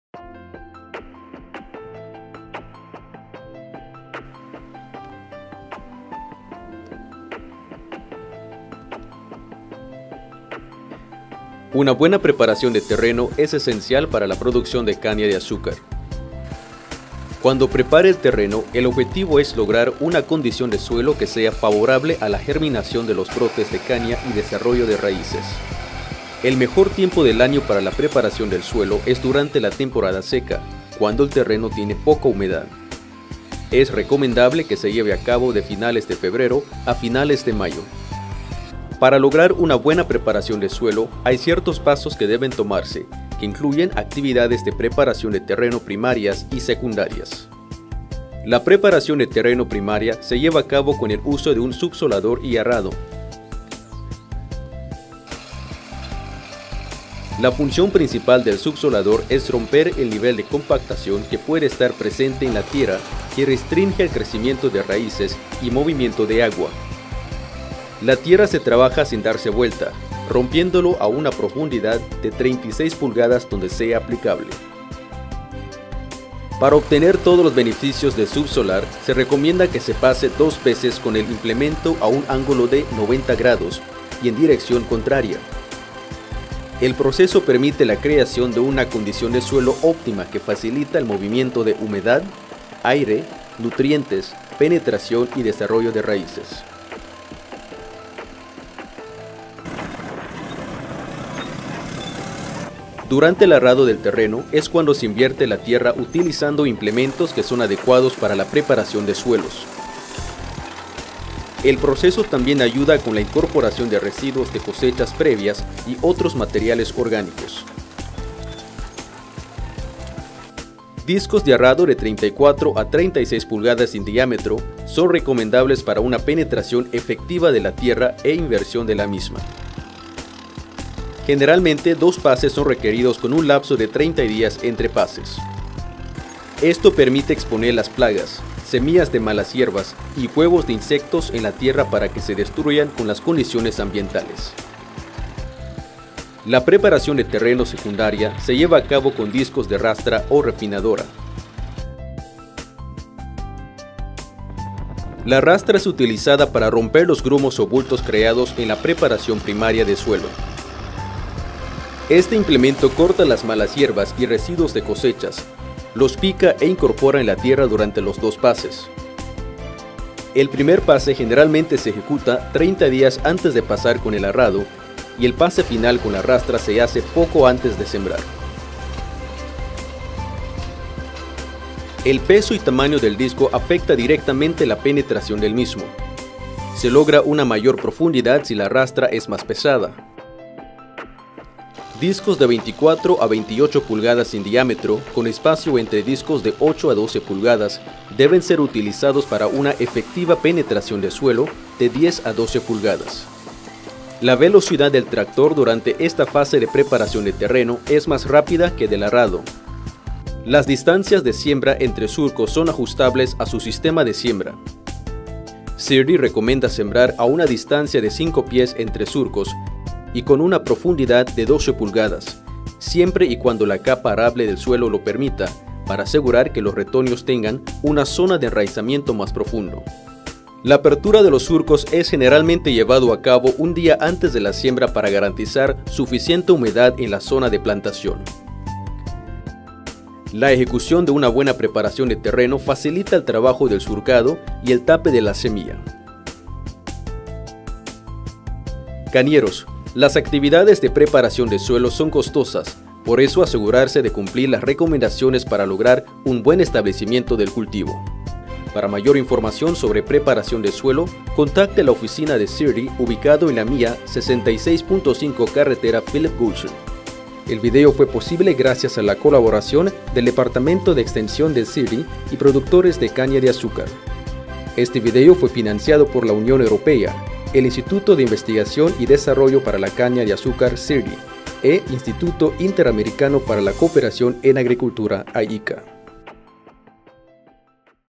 SIRDI - Land preparation documentary
Belize's Sugar Industry Research and Development Institute has created a documentary to help sugar cane farmers adapt to best management practices for land preparation.